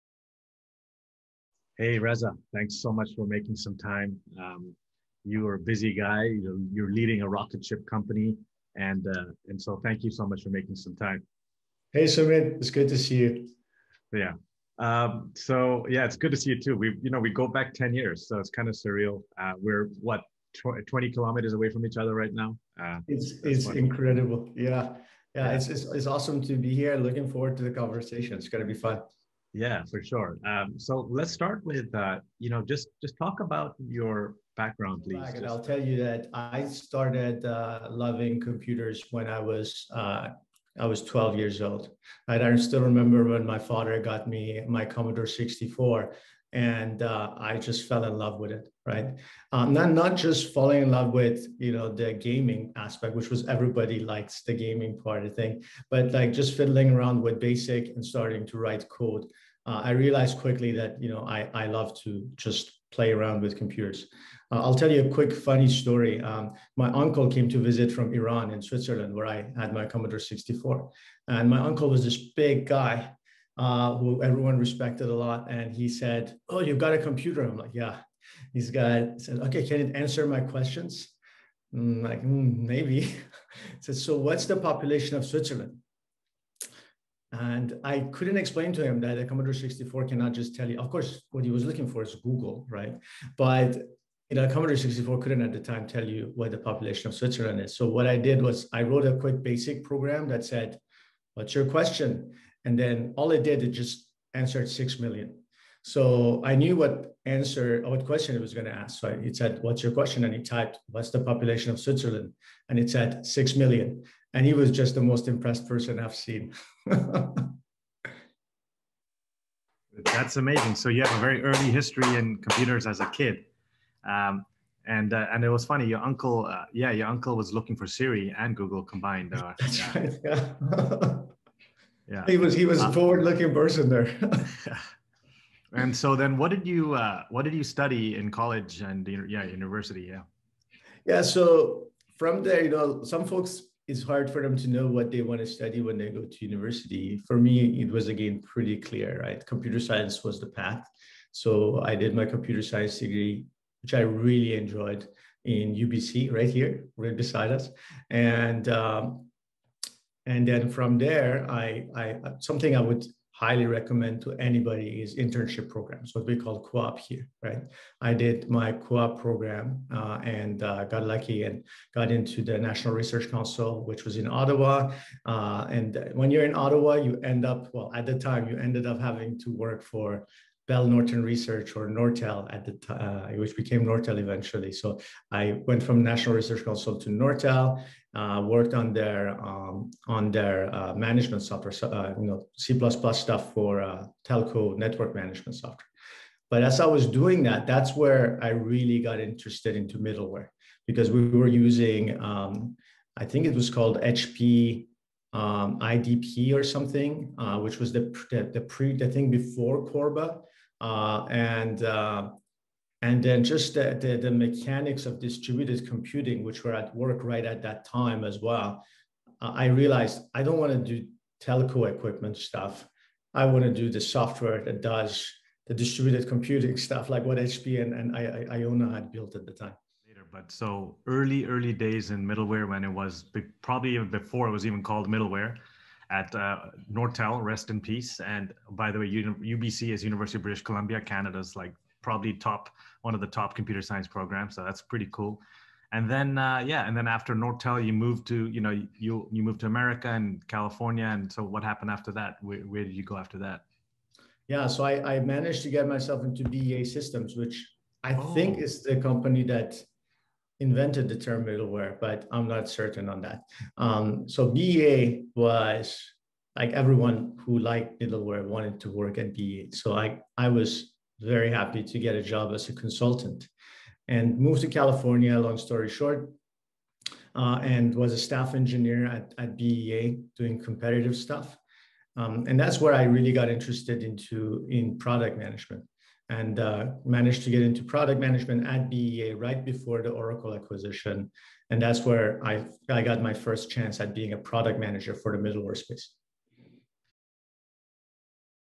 Reza Shafii - Early Years
Reza is the VP of Products at Kong, one of the fastest growing enterprise software unicorns which just recently raised $100M from funds such as Andreessen Horowitz, Goldman Sachs and Tiger Global amongst others.

Reza is one of the world’s leading authorities on APIs, integration, connectivity and cloud native technology.

Before Kong, Reza led Products at CoreOS (the container platform and Kubernetes pioneer company) which was acquired by Red Hat where he continued on as VP.

Prior to Red Hat, Reza helped build MuleSoft’s API Platform product suite from ground zero, resulting in MuleSoft achieving unicorn status (and the rest is history as they say).

Prior to MuleSoft, Reza worked within Thomas Kurian’s organization (now CEO of Google Cloud) at Oracle leading the product management of various Oracle platform products.